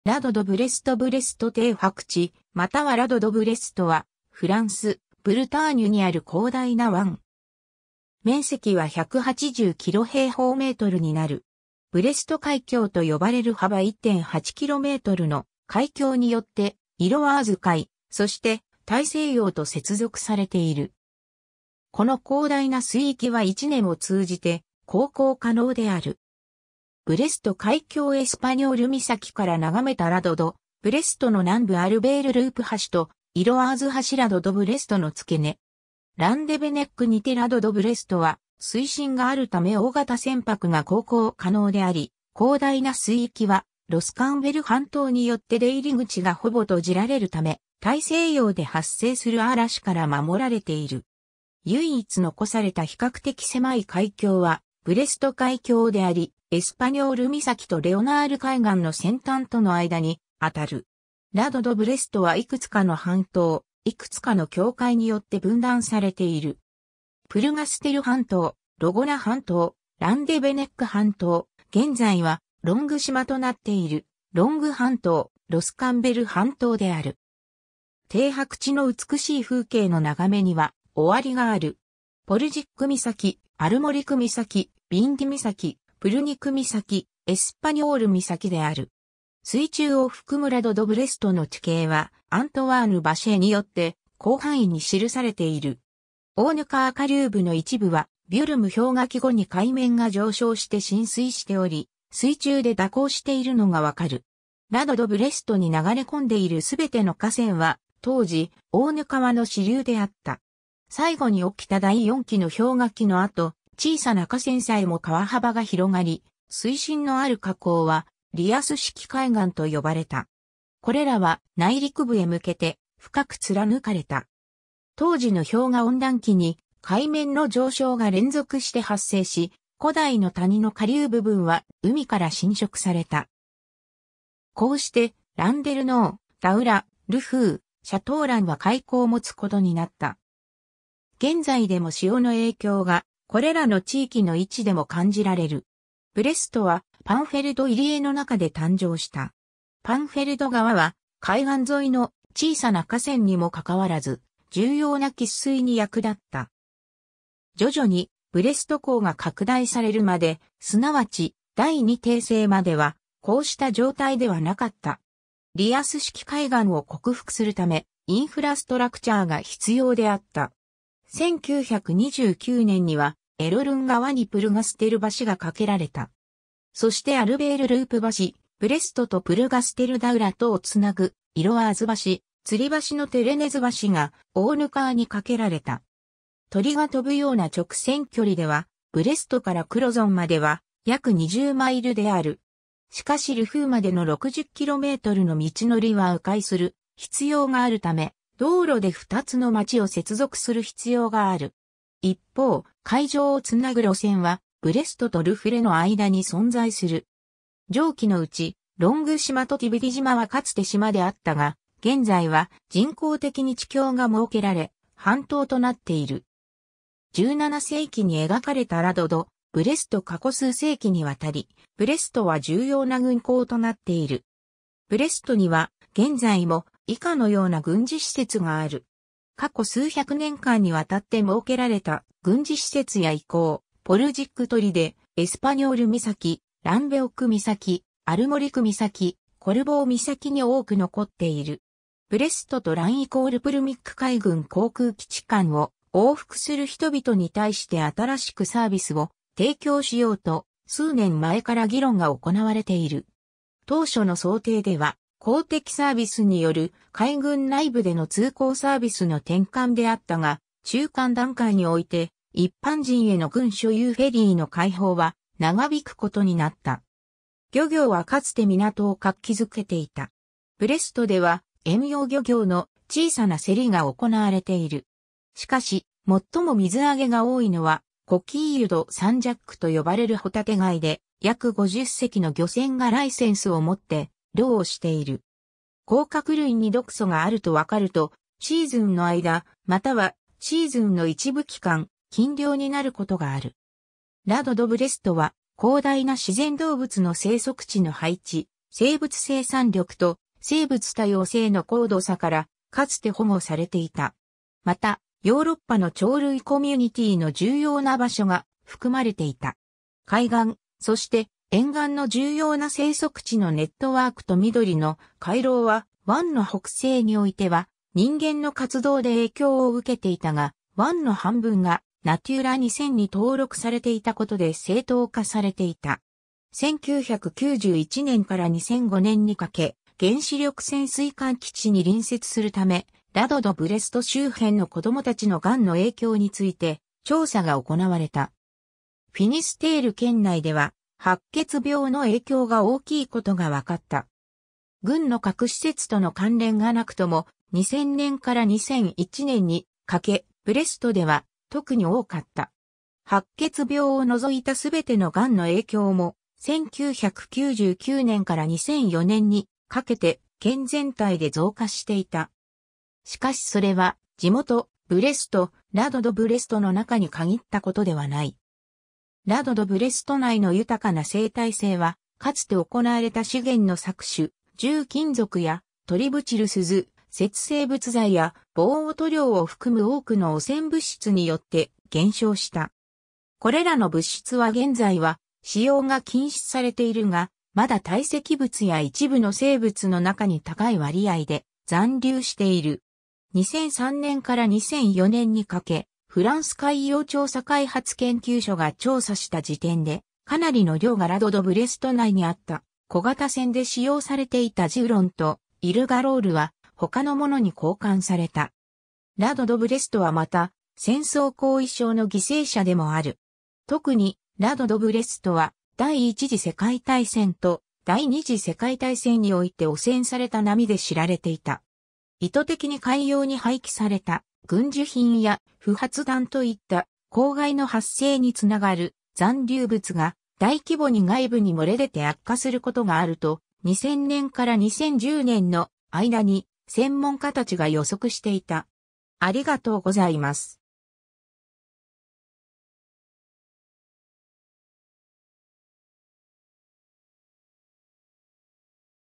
ブレスト停泊地
ラド・ド・ブレスト ブレスト停泊地 （ブレストていはくち）またはラド・ド・ブレスト（Rade de Brest、ブルトン語:Lenn-vor Brest）は、フランス、ブルターニュにある広大な湾。
面積は180 km²になる。
ブレスト海峡と呼ばれる幅1.8kmの海峡によってイロワーズ海、そして大西洋と接続されている。
この広大な水域は一年を通じて航行可能である。
 ブレスト海峡 エスパニョール岬から眺めたラド・ド・ブレストの南部 アルベール・ループ橋とイロワーズ橋 ラド・ド・ブレストの付け根。
ランデヴェネックにて ラド・ド・ブレストは水深があるため大型船舶が航行可能であり、広大な水域はロスカンヴェル半島によって出入り口がほぼ閉じられるため、大西洋で発生する嵐から守られている。
唯一残された比較的狭い海峡はブレスト海峡であり、エスパニョール岬とレオナール海岸の先端との間にあたる。
 ラド・ド・ブレストはいくつかの半島、いくつかの境界によって分断されている。
プルガステル半島、ロゴナ半島、ランデヴェネック半島、現在はロング島となっているロング半島、ロスカンヴェル半島である。
停泊地の美しい風景の眺めには終わりがある。
ポルジック岬、アルモリク岬、ビンディ岬、プルニク岬、エスパニョール岬である。
 水中を含むラド・ド・ブレストの地形は、アントワーヌ・ヴァシェによって広範囲に記されている。
オーヌ川下流部の一部は、ヴュルム氷河期後に海面が上昇して浸水しており、水中で蛇行しているのがわかる。
ラド・ド・ブレストに流れ込んでいる全ての河川は、当時オーヌ川の支流であった。
 最後に起きた第四紀の氷河期のあと、小さな河川さえも川幅が広がり、水深のある河口はリアス式海岸と呼ばれた。
これらは内陸部へ向けて深く貫かれた。
当時の氷河温暖期に海面の上昇が連続して発生し、古代の谷の下流部分は海から侵食された。
こうしてランデルノー、ダウラ、ル・フー、シャトーランは海港を持つことになった。
現在でも潮の影響が、これらの地域の位置でも感じられる。
ブレストはパンフェルド入江の中で誕生した。
パンフェルド川は海岸沿いの小さな河川にもかかわらず、重要な喫水に役立った。
徐々にブレスト港が拡大されるまで、すなわち第二帝政までは、こうした状態ではなかった。
 リアス式海岸を克服するため、インフラストラクチャーが必要であった。
1929年にはエロルン川にプルガステル橋が架けられた。
そしてアルベール・ループ橋、ブレストとプルガステル＝ダウラとをつなぐイロワーズ橋、吊り橋のテレネズ橋がオーヌ川に架けられた。
鳥が飛ぶような直線距離では、ブレストからクロゾンまでは約20マイルである。
しかしル・フーまでの60kmの道のりは迂回する必要があるため、道路で2つの町を接続する必要がある。
一方、海上をつなぐ路線はブレストとル・フレ（クロゾン）の間に存在する。
 上記のうち、ロング島とティビディ島はかつて島であったが、現在は人工的に地峡が設けられ半島となっている。
 17世紀に描かれたラド・ド・ブレスト 過去数世紀にわたり、ブレストは重要な軍港となっている。
ブレストには現在も以下のような軍事施設がある。
 過去数百年間にわたって設けられた軍事施設や遺構、ポルジック砦、エスパニョール岬、ランヴェオック岬、アルモリク岬、コルボー岬に多く残っている。
 ブレストとランヴェオック＝プルミック海軍航空基地間を往復する人々に対して新しくサービスを提供しようと、数年前から議論が行われている。
当初の想定では、公的サービスによる海軍内部での通行サービスの転換であったが、中間段階において、一般人への軍所有フェリーの開放は長引くことになった。
 漁業はかつて港を活気づけていた。
ブレストでは遠洋漁業の小さな競りが行われている。
しかし、最も水揚げが多いのは、コキーユ・ド・サン＝ジャックと呼ばれるホタテガイで、約50隻の漁船がライセンスを持って漁をしている。
甲殻類に毒素があるとわかると、シーズンの間、またはシーズンの一部期間、禁漁になることがある。
 ラド・ド・ブレストは、広大な自然動物の生息地の配置、生物生産力と生物多様性の高度さからかつて保護されていた。
また、ヨーロッパの鳥類コミュニティーの重要な場所が含まれていた。
海岸、そして沿岸の重要な生息地のネットワークと緑の回廊は、湾の北西においては人間の活動で影響を受けていたが、湾の半分がナテュラ2000（fr）に登録され...